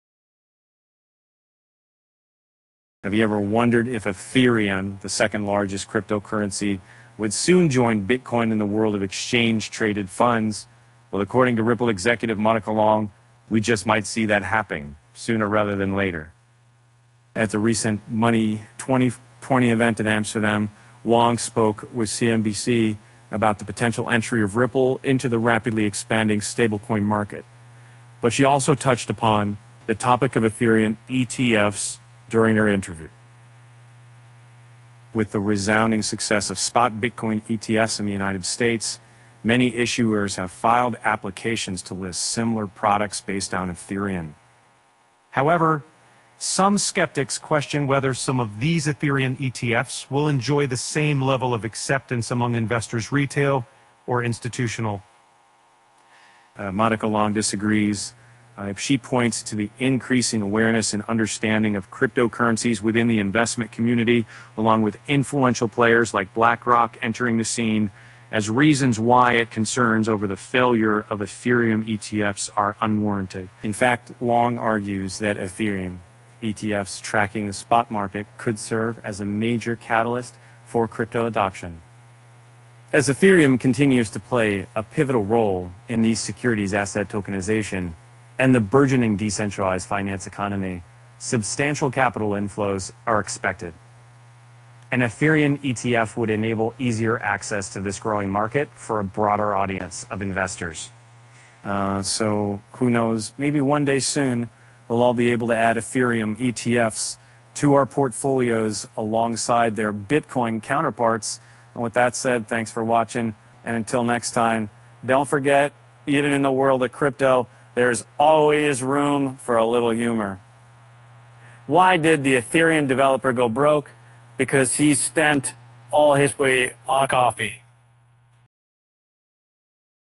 Ripple president: Stablecoin likely this year
Have you ever wondered if Ethereum, the second largest cryptocurrency, would soon join Bitcoin in the world of Exchange Traded Funds (ETFs)? Well, according to Ripple executive Monica Long, we just might see that happen sooner rather than later.